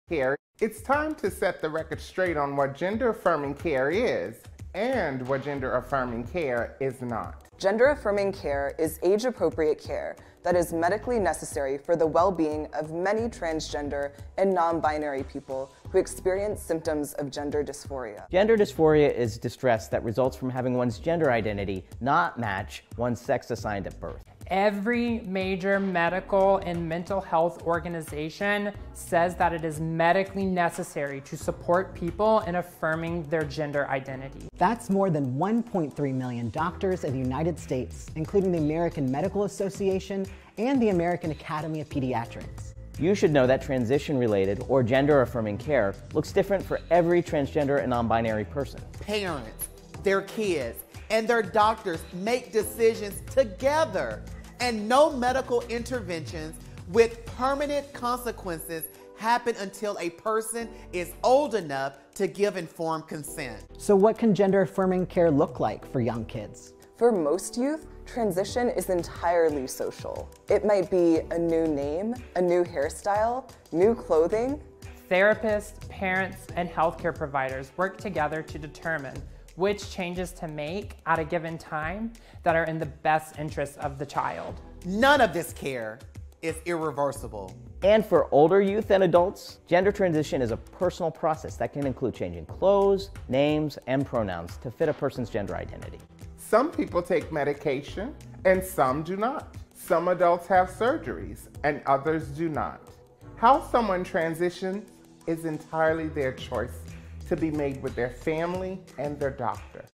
TransClinique : Gender Affirming Care Saves Lives ( Book Your free Consultation Today )
Healthcare delivered via telemedicine has important advantages for trans and non-binary folks who may not have easy access to knowledgeable and safe providers near their homes. When possible, our healthcare providers identify as trans, non-binary, or queer themselves, allowing for more comfortable visits discussing issues that are often difficult with other healthcare providers.

Here’s how our process is broken down:

Step One - Initial Free Consult
You’ll meet with your provider to go over intake forms, informed consent and cover any questions/concerns you may have. At the end of this meeting, you’ll be given a lab panel request.

Step Two - Laboratory Tests
Complete your lab panel, and once your blood work results are sent back, we will check your hormone levels, liver and kidney function before starting any HRT. This ensures we provide the best care in the safest way possible for you.

Step Three - Hormone Therapy
Once your full medical evaluation is complete and you feel ready to start, we’ll write and fill your prescription. Then, we provide you with easy-to-follow steps on how to proceed.

We strive to help you become your most authentic self through full-service monthly subscription plans that include all necessary lab tests, prescribing of HRT medications, access to weekly educational seminars, and live virtual fitness classes.